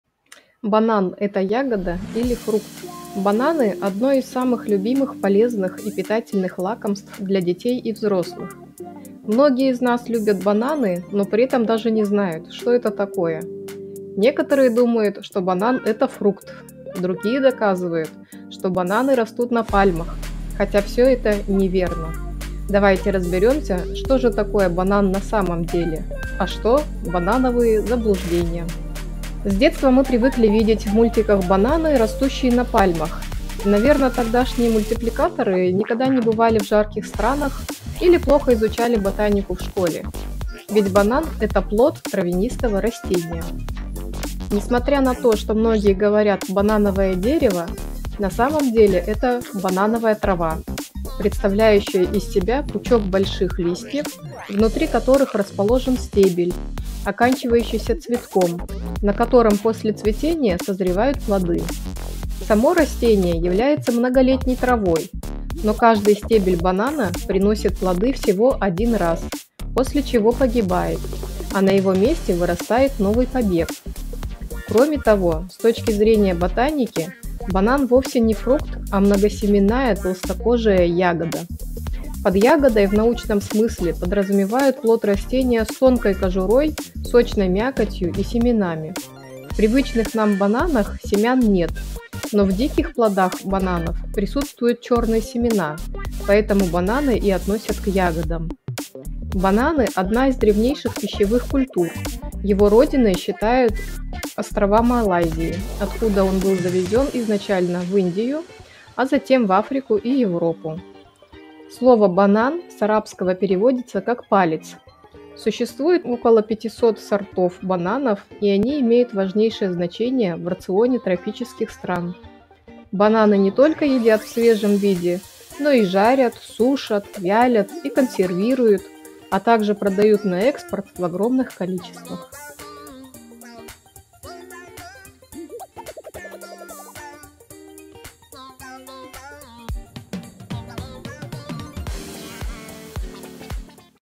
Банан - это ягода или фрукт? |Занимательные факты| Еда
Разбираемся, что мы едим. Чем является банан на самом деле? Вроде очевидные вещи, но всё не так просто!) Приятного прослушивания! Понравилась история?
Обязательно комментируйте, а что вы думаете по поводу этой истории?